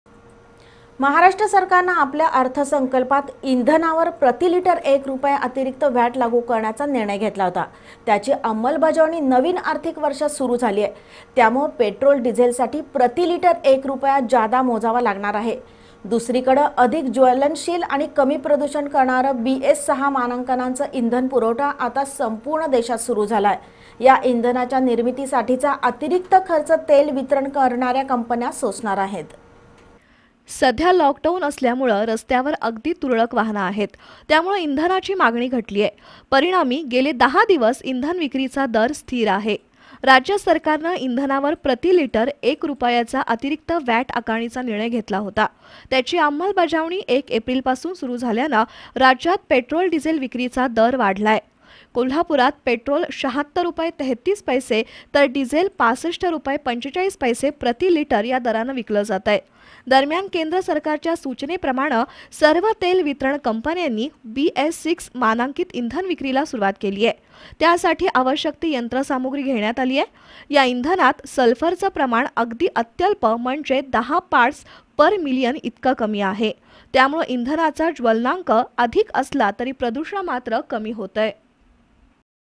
राज्यात पेट्रोल-डिझेल प्रती लिटर १ रूपयानं महागले
राज्यात पेट्रोल-डिझेल प्रती लिटर १ रूपयानं महागले, तरी कर्नाटकपेक्षा महाराष्ट्रात डिझेल स्वस्त, बीएस-६ इंधन पुरवठयालाही सुरूवात
Channel B